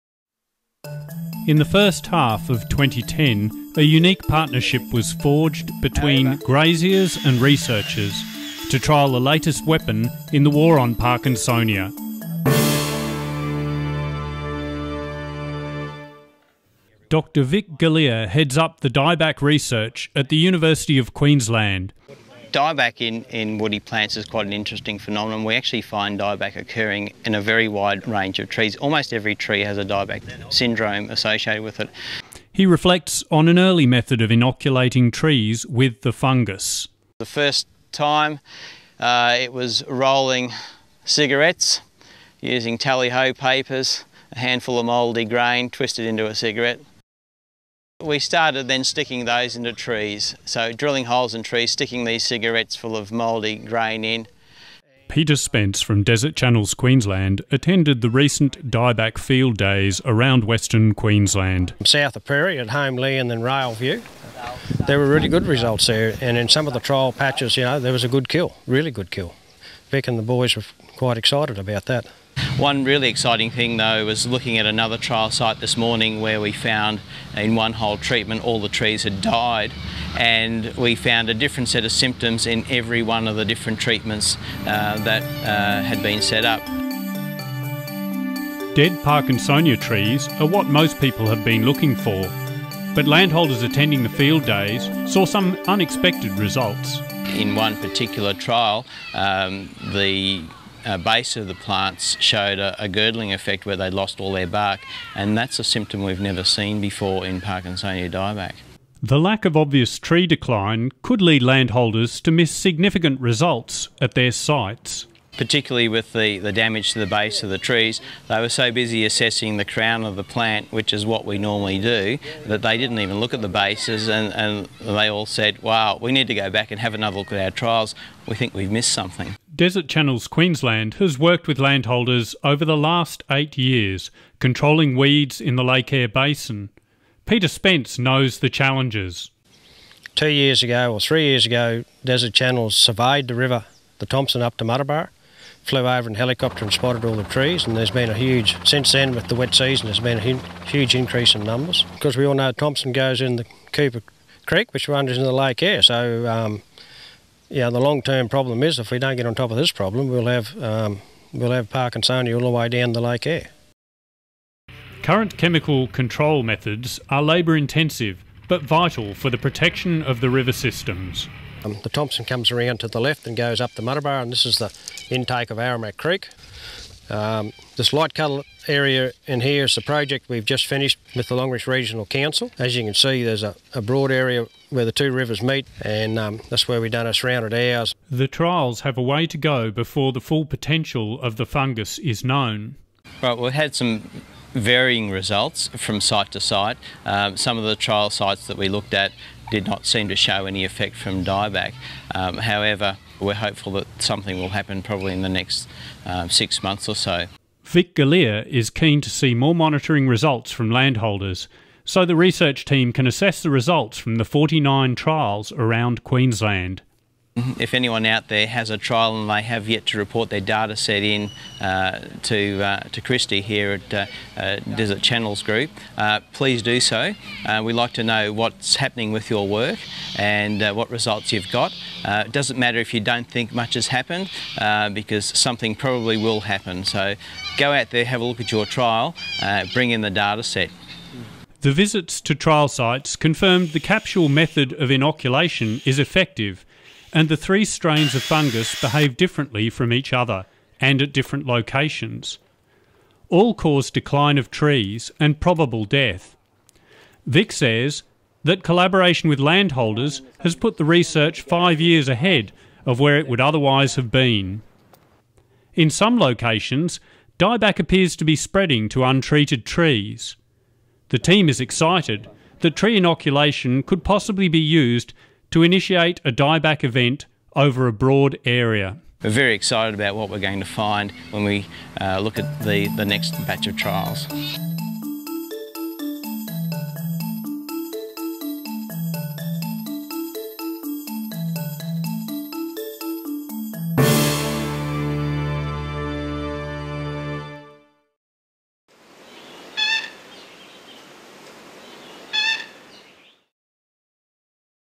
DCQ News Update - Parkinsonia Dieback - November 2011
Trials of a dieback fungus for Parkinsonia are showing great promise. Field days, conducted around the Western Queensland trial sites, have excited landholders and researchers as they discovered unexpected results. Vic Galea from the University of Queensland, heads up the research project.